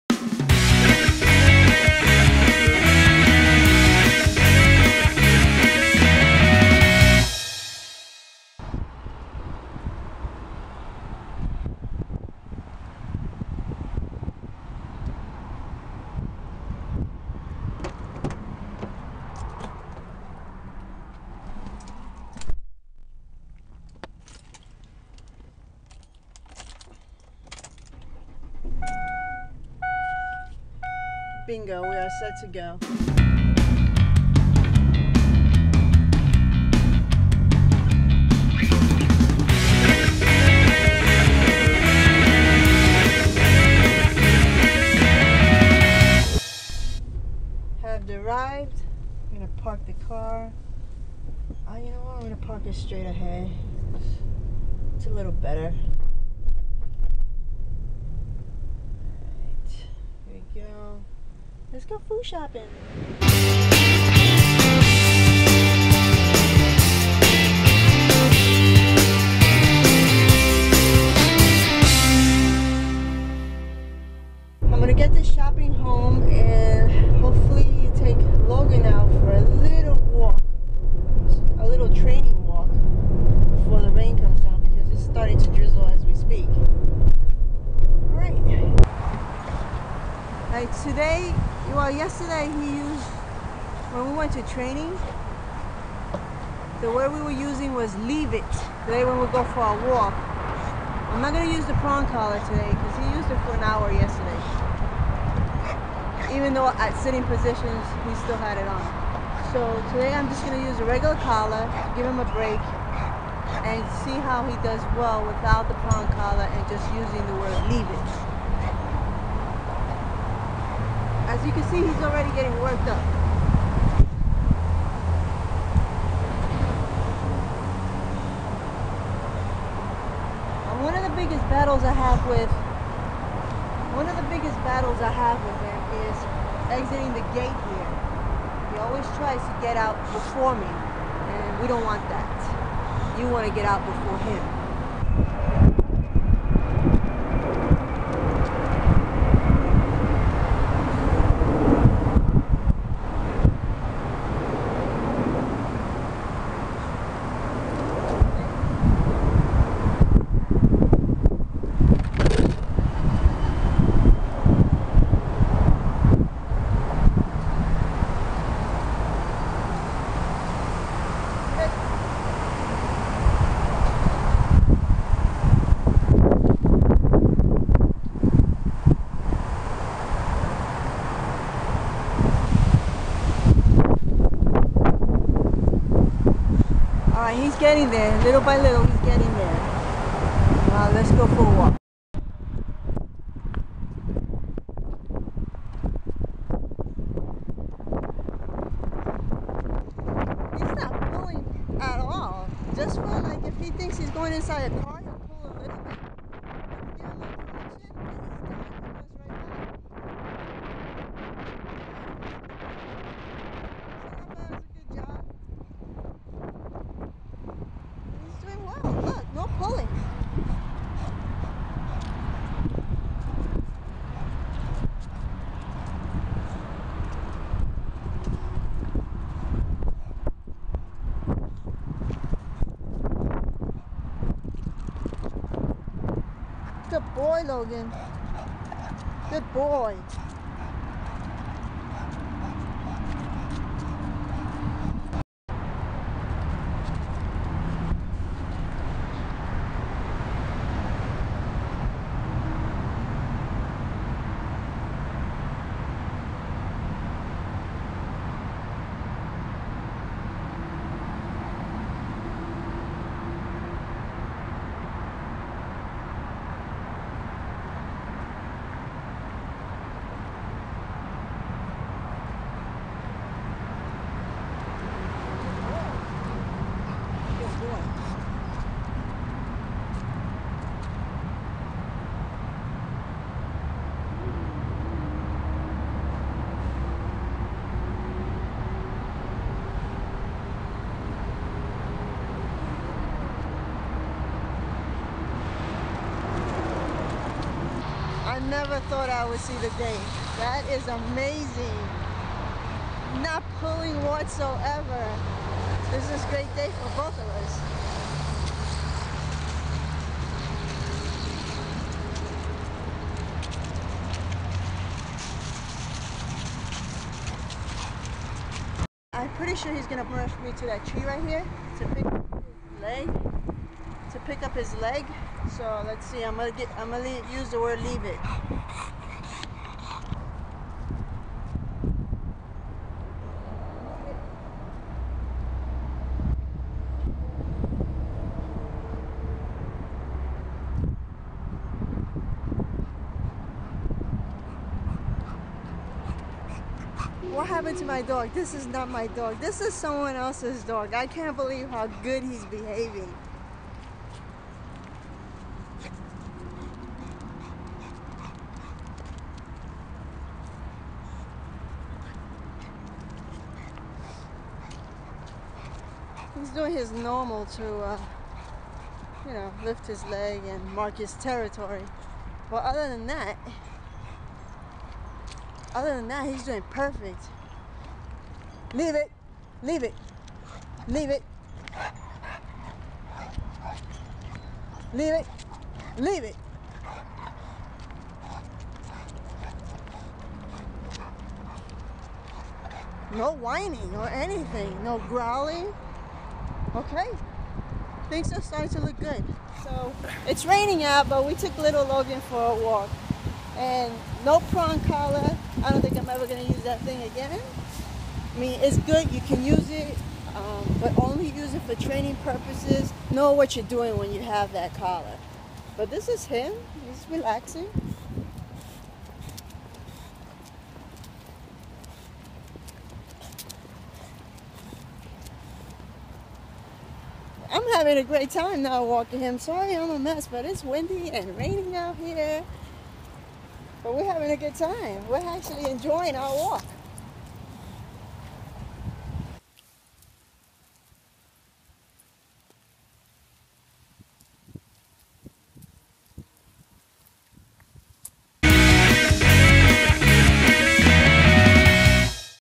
Logan our bulldog is making strides in just one week of training
This video is about How to Train you bulldog puppy. Join me as I train my aggressive bulldog Logan into a calm pack member..